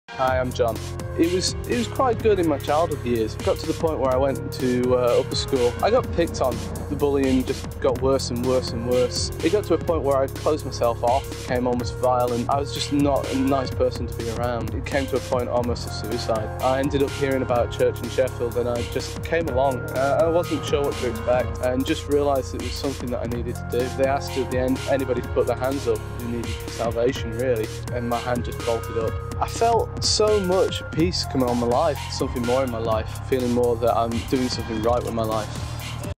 Existence - John
Fast paced testimonies of how Jesus Christ makes the difference in our existence. Hear from young people firsthand who have been saved, healed, or set free by the power of God.